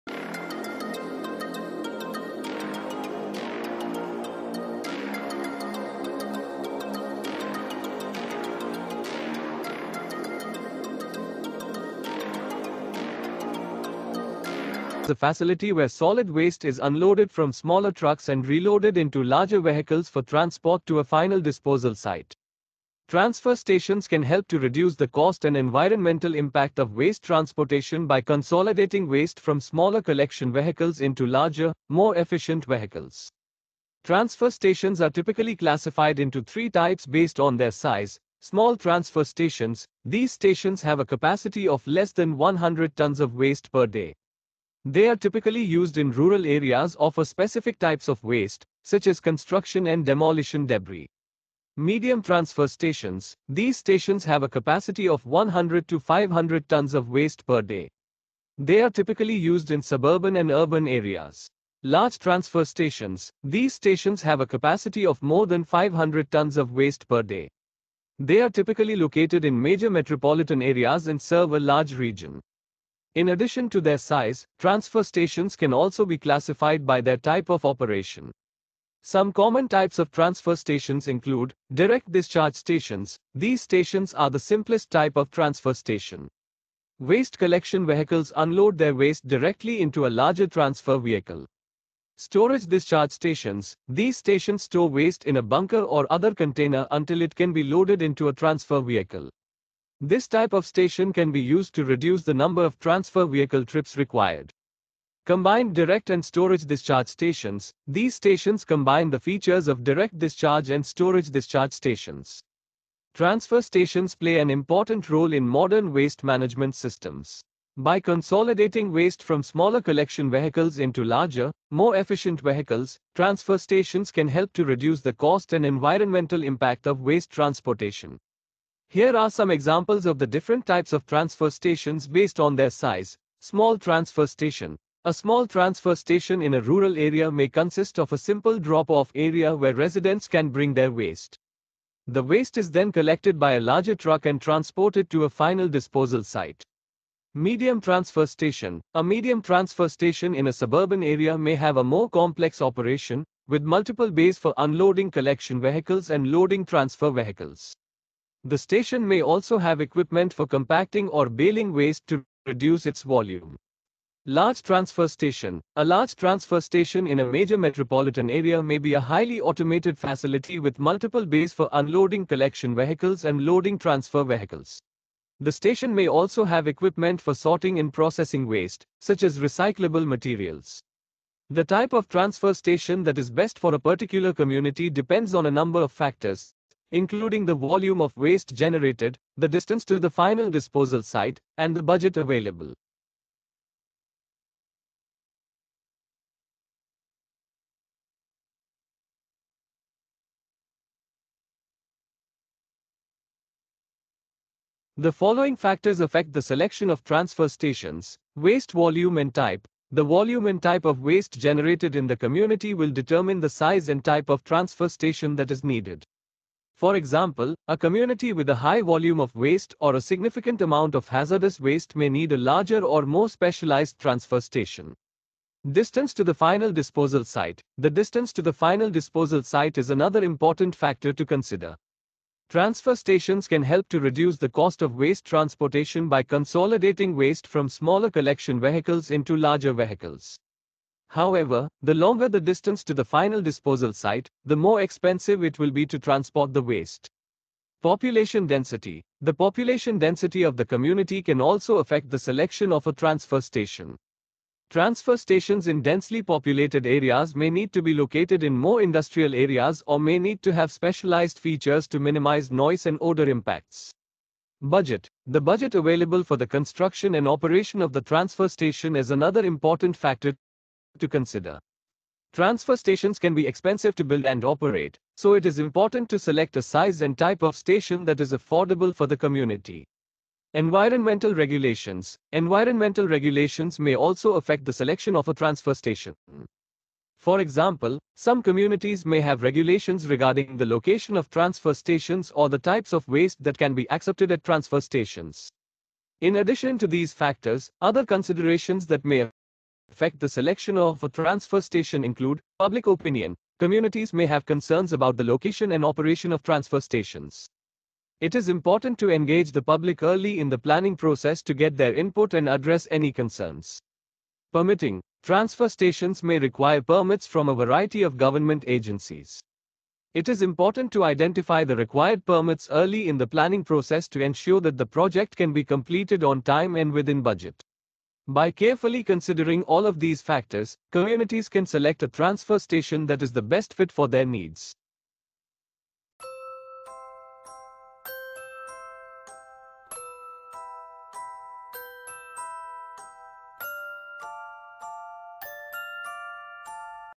20 What is a Transfer station Explain its types based on its size
Title: Demystifying Transfer Stations: Types and Significance | BETCK205F/BETCK105F Insights

Description:
Unravel the concept of Transfer Stations and explore their various types based on size in this informative session for BETCK205F/BETCK105F. 🌐🏗️ Gain a comprehensive understanding of these crucial waste management facilities.

🔥 Explore Transfer Stations: [Link to YouTube Video Goes Here] 🔥

A compelling visual featuring a transfer station with various waste collection components in action. Overlay text reads "Transfer Stations: Types and Significance" to capture attention.

Embark on this enlightening journey to understand the critical role of Transfer Stations in waste management. Whether you're a student or an enthusiast, this video empowers your understanding.